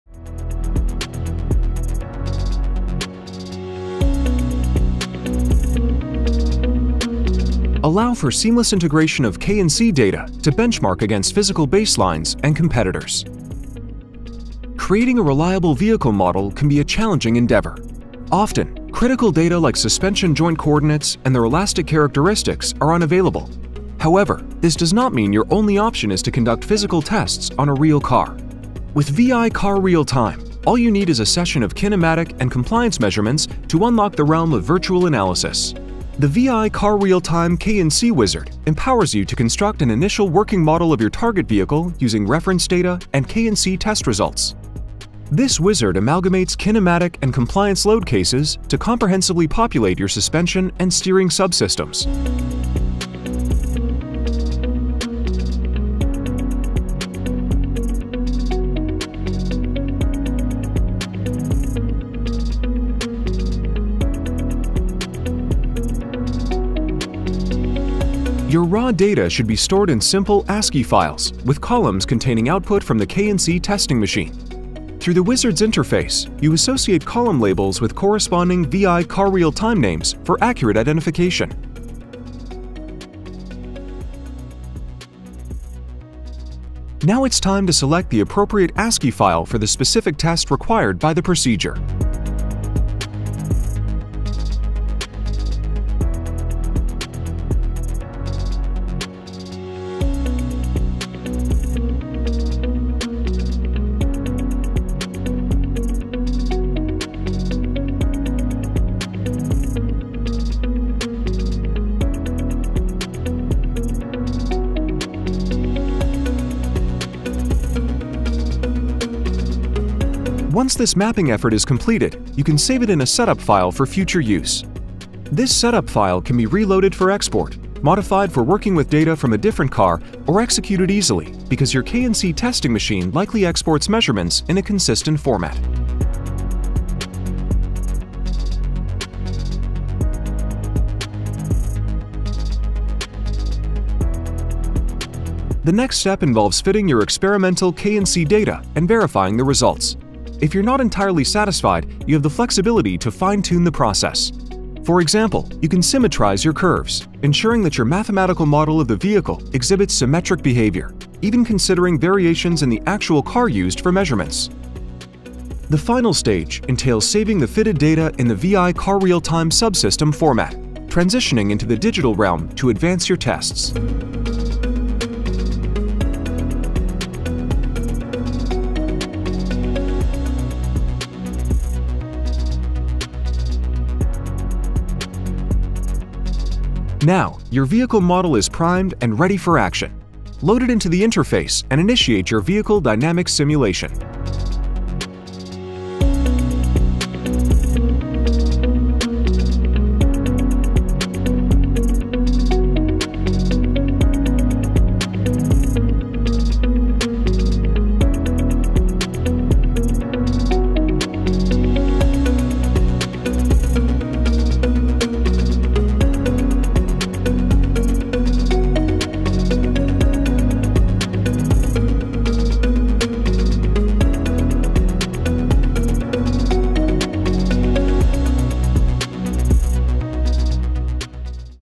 VI-CarRealTime Video Tutorial #2 - Seamless integration of K&C data to benchmark against baselines
Creating a reliable vehicle model can be a challenging endeavor. Often, critical data like suspension joint coordinates and their elastic characteristics are unavailable. However, this does not mean your only option is to conduct physical tests on a real car.

With VI-CarRealTime, all you need is a session of kinematic and compliance measurements to unlock the realm of virtual analysis.

**The VI-CarRealTime K&C Wizard** empowers you to construct an initial working model of your target vehicle using reference data and K&C test results.

This wizard amalgamates kinematic and compliance load cases to comprehensively populate your suspension and steering subsystems. Your raw data should be stored in simple ASCII files, with columns containing output from the K&C testing machine. Through the Wizard's interface, you associate column labels with corresponding VI-CarRealTime names for accurate identification.

Now, it's time to select the appropriate ASCII file for the specific test required by the procedure.

Once this mapping effort is completed, you can save it in a setup file for future use. This setup file can be reloaded for export, modified for working with data from a different car, or executed easily because your K&C testing machine likely exports measurements in a consistent format.

The next step involves fitting your experimental K&C data and verifying the results. If you're not entirely satisfied, you have the flexibility to fine-tune the process. For example, you can symmetrize your curves, ensuring that your mathematical model of the vehicle exhibits symmetric behavior, even considering variations in the actual car used for measurements.

The final stage entails saving the fitted data in the VI-CarRealTime subsystem format, transitioning into the digital realm to advance your tests.

Now, your vehicle model is primed and ready for action. Load it into the interface and initiate your vehicle dynamics simulation.

For more information on VI-CarRealTime or to request your demo license